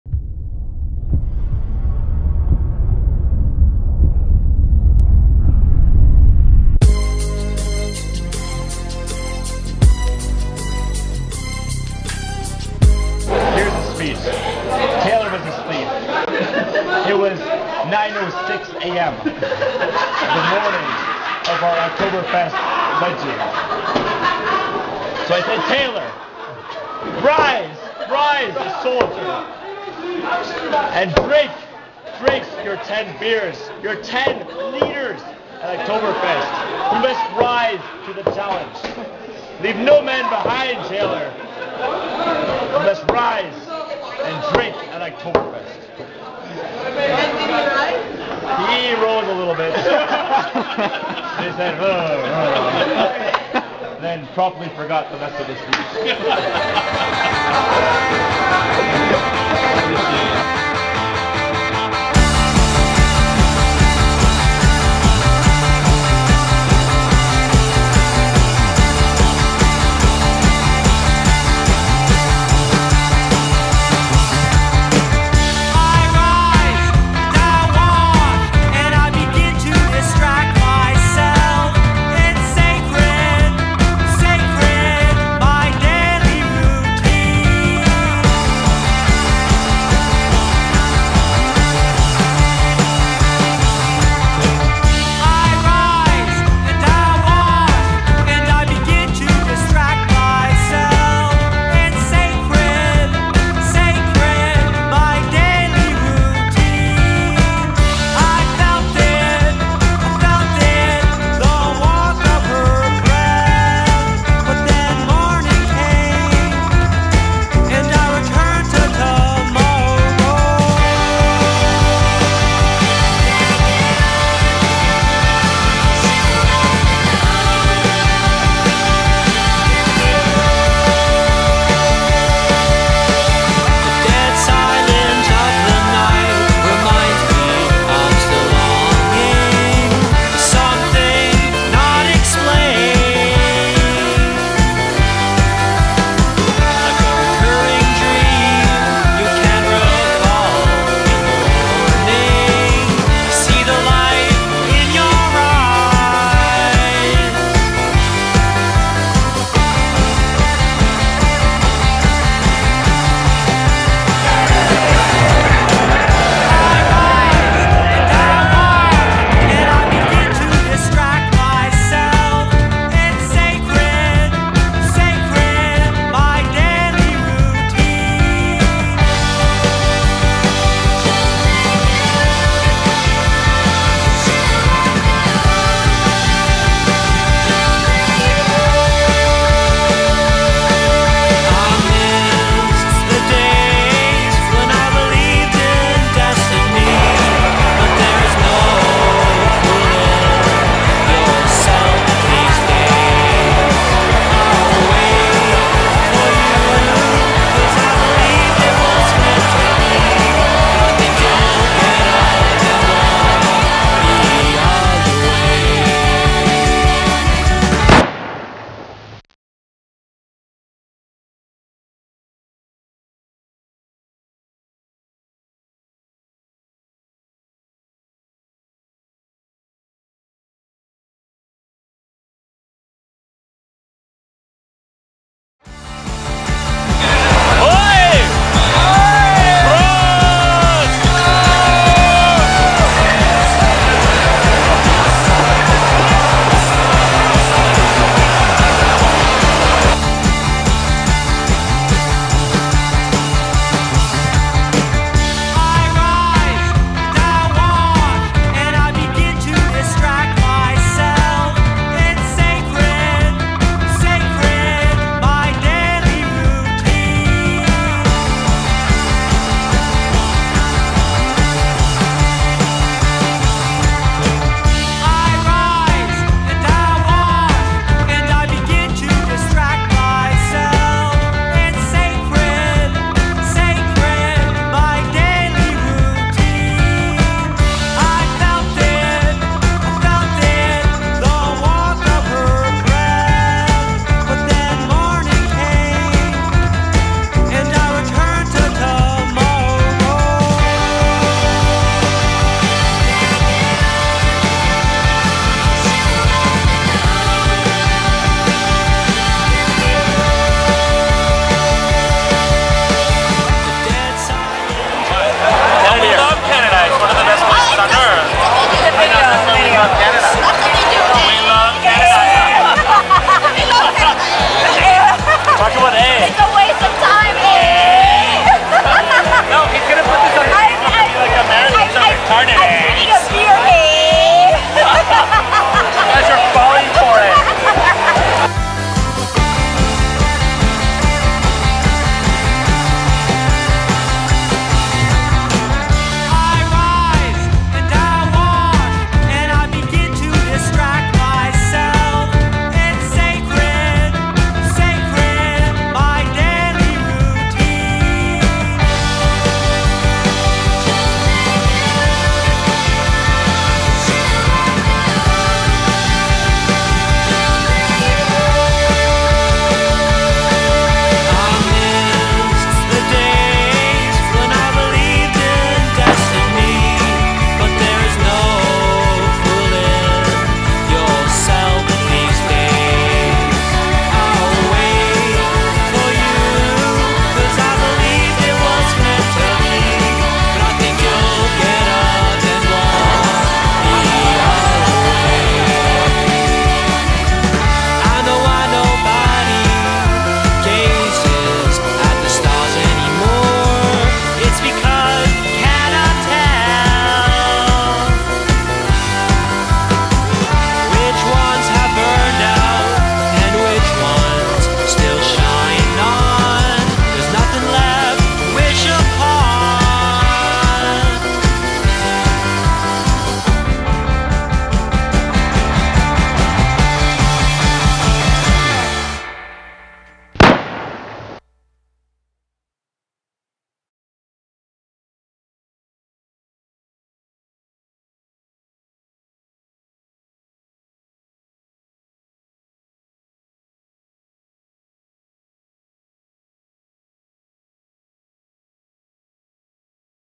Oktoberfest 2009: The Film - Chapter 6 - The Battle of Oktoberfest
This is the story of a group of Canadians who went to Oktoberfest and how we got there.  It was cobbled together from whatever footage survived on our respective cameras.